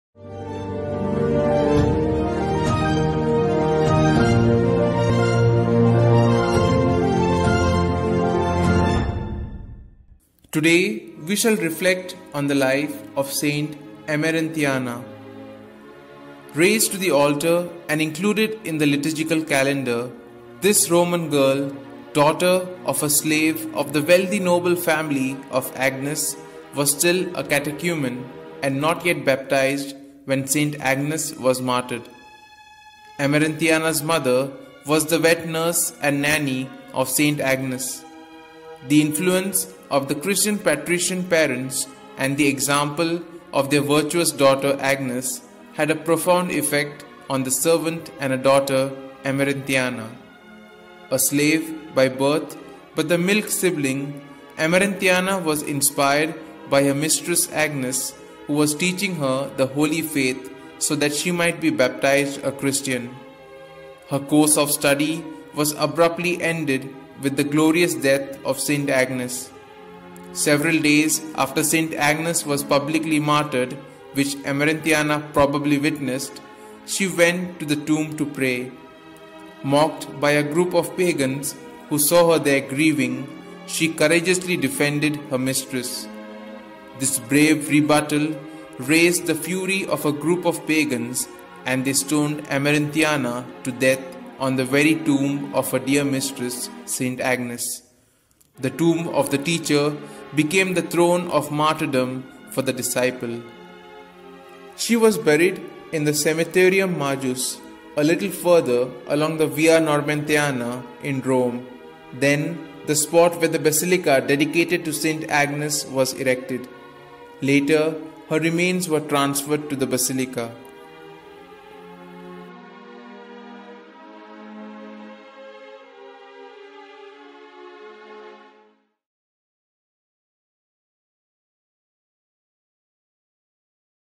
St Emerentiana - Saint of the Day with Fr Lindsay - 23 January 2022
Here is a brief reflection on the life of St Emerentiana presented by Fr Lindsay Lobo.